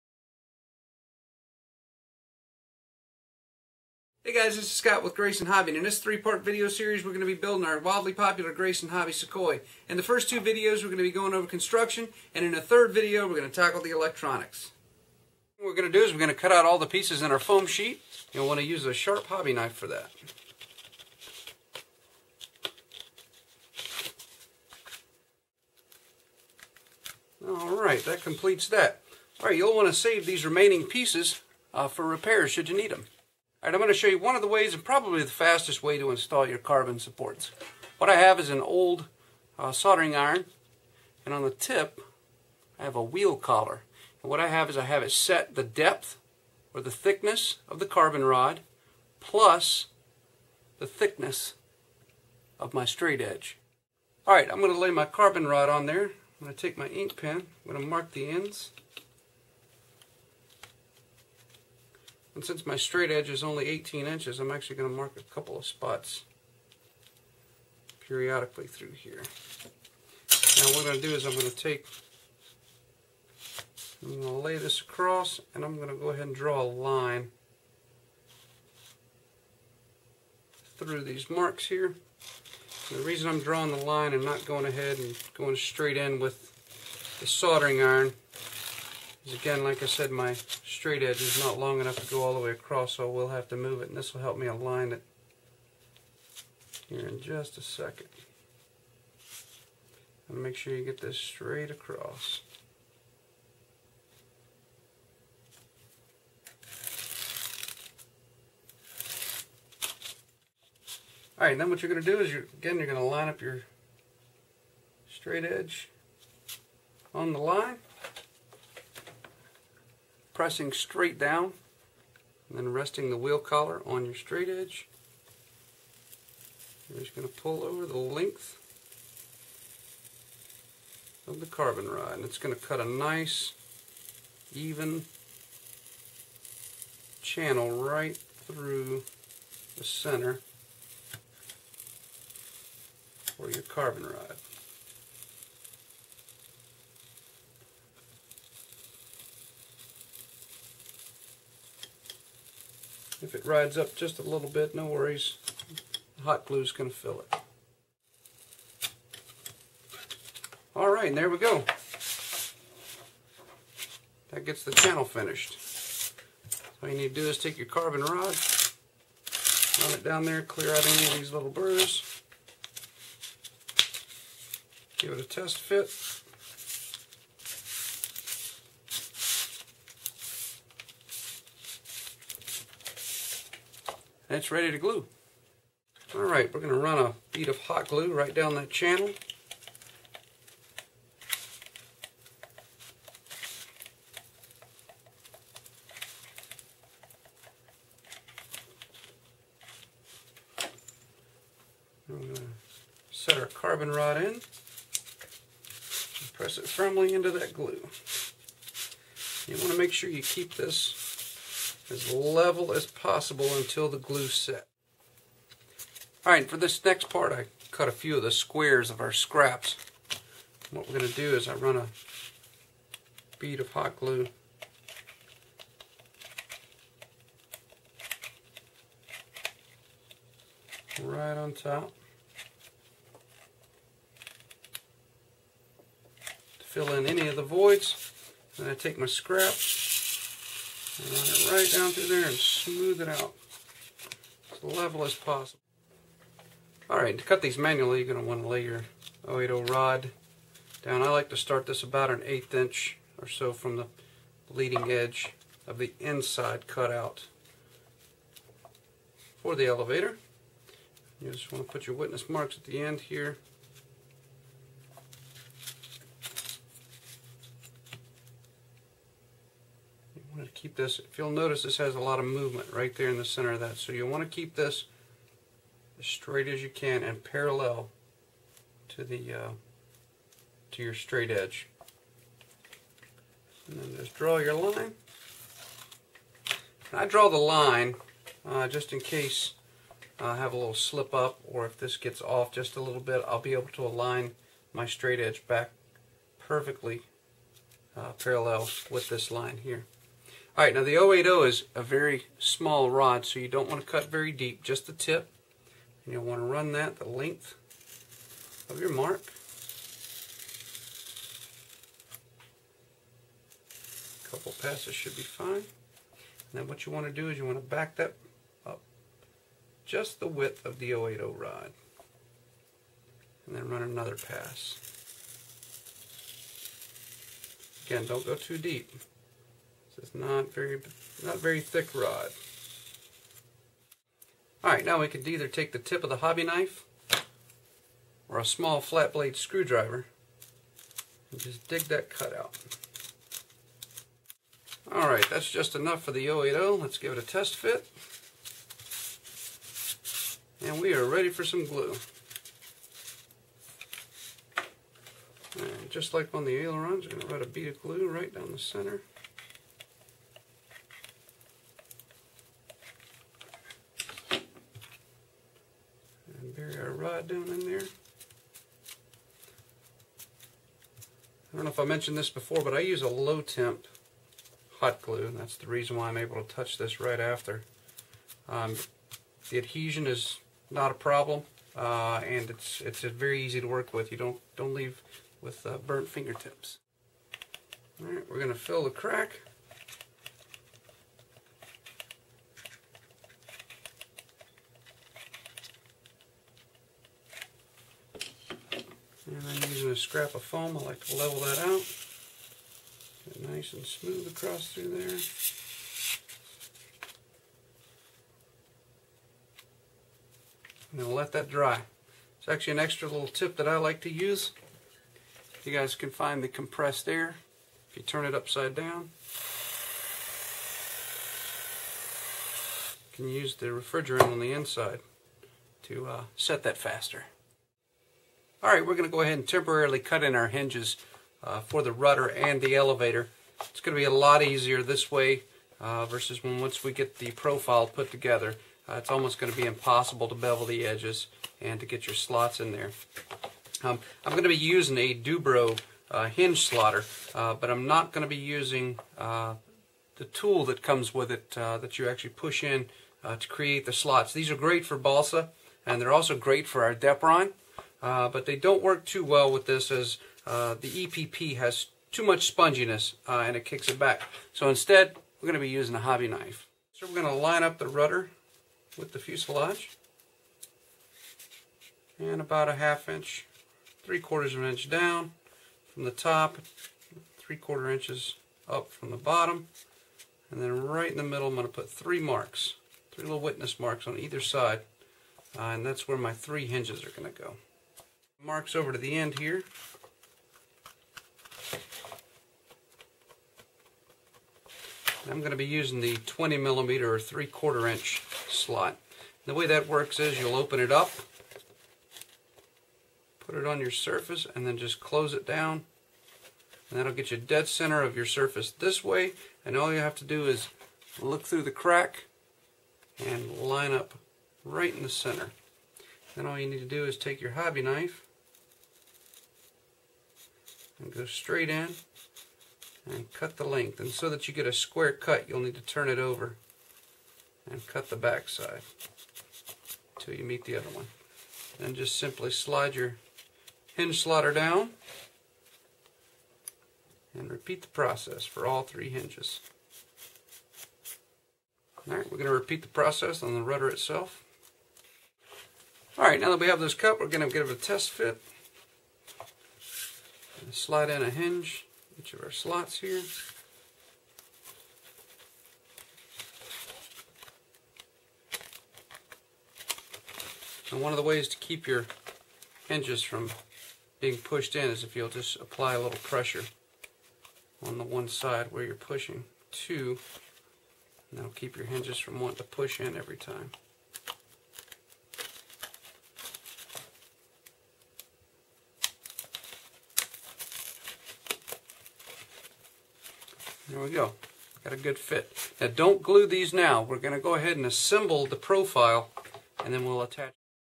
Sukhoi Build 1
Part one of our Grayson Hobby Sukhoi build, construction. Be sure to check out the second in this series and the third for the installation of the electronics.